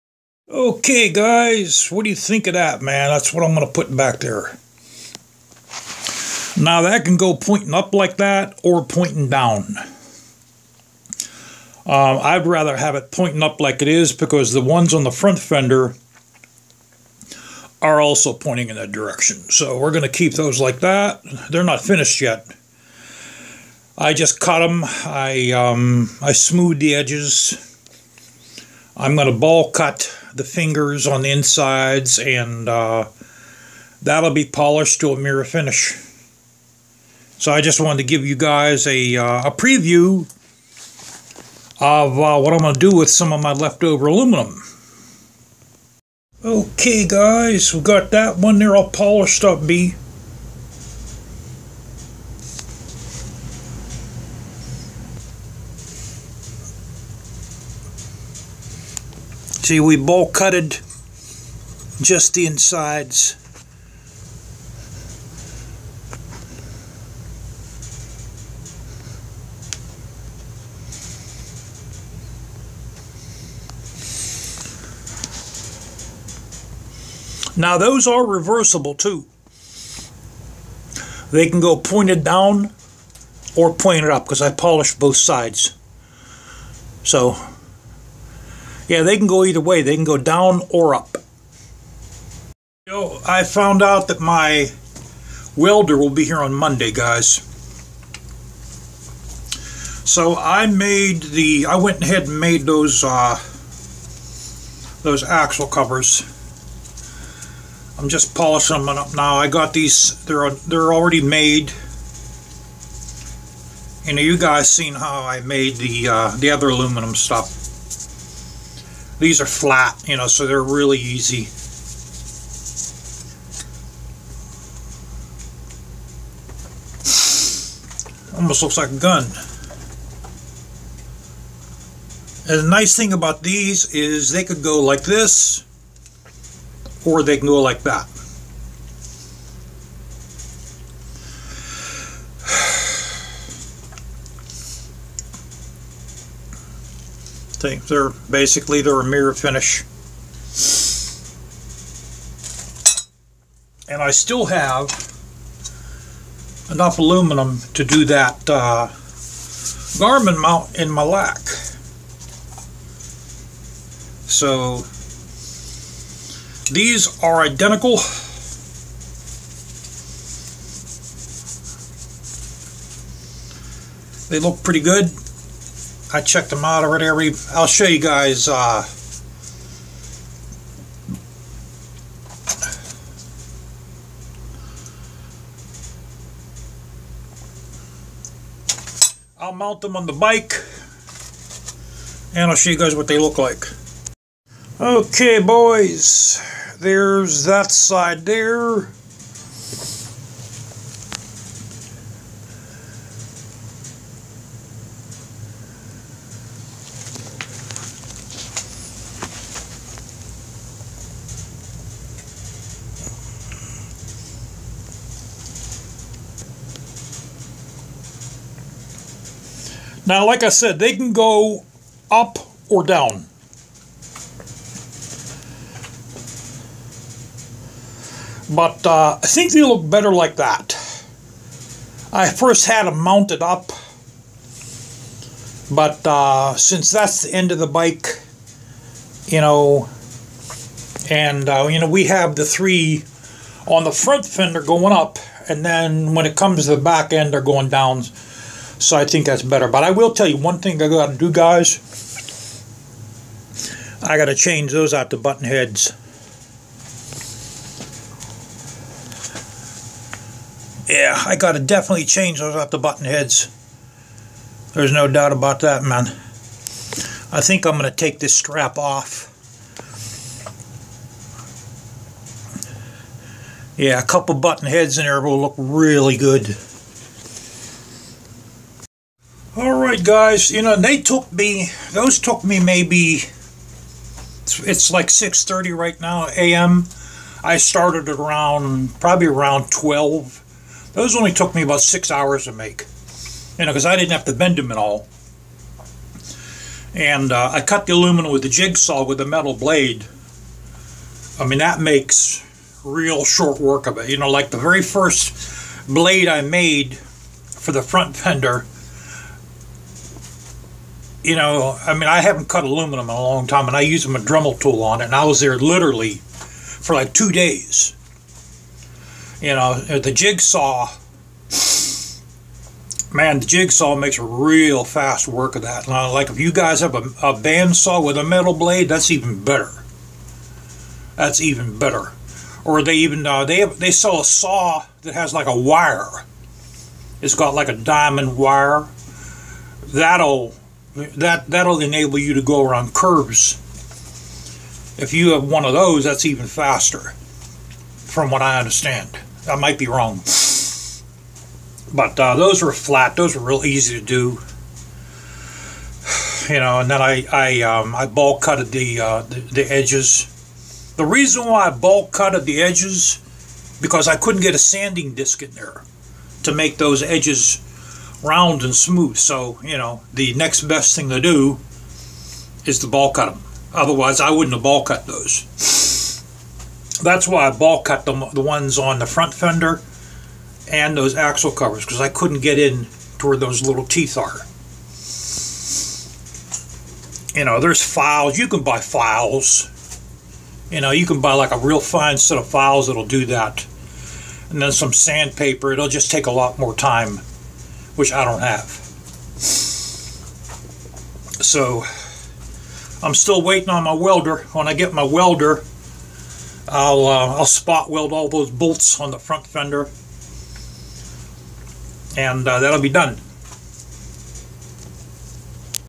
Hand made aluminum axle covers
I made these solely by hand. Tools used. Jigsaz, dremel,vise,drill. Sandpaper,240,400,800,1000,2000,3000,5000,7000. Buffing wheel and green dialux compound.And 3m aluminum polish.